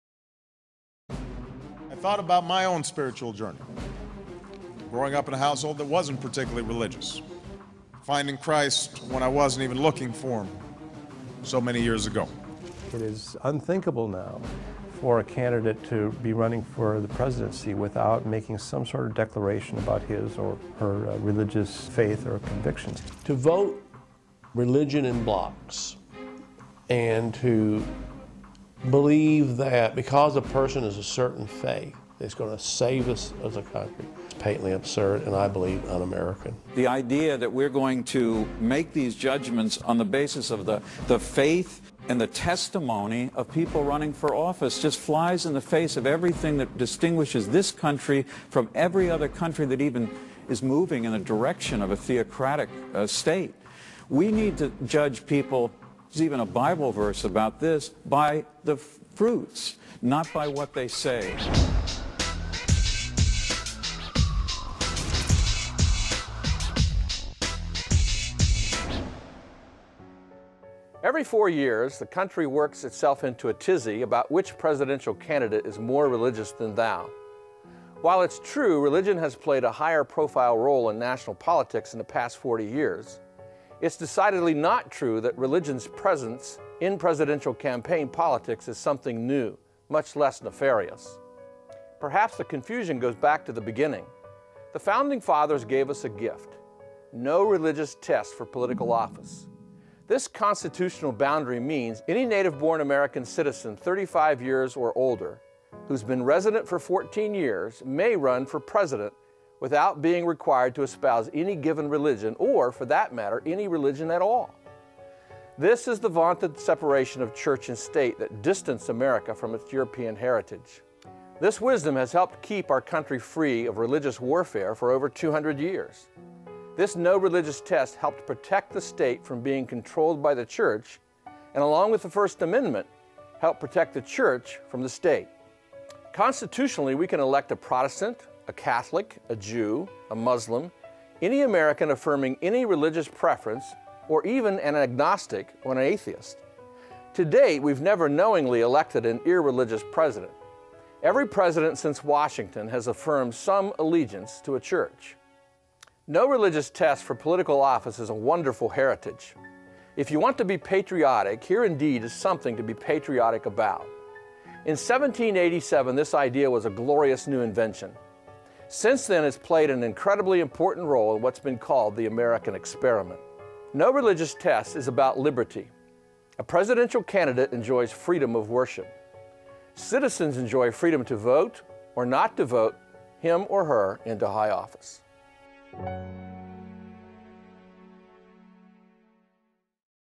Making a Difference: Should Religion be a Qualification for the Presidency?
Our Founding Fathers gave us a gift, the gift of No Religious Test for a president. This gift gives presidential candidates the freedom of worship, and it gives us, the citizens, the freedom to vote, or not to vote him or her into higher office.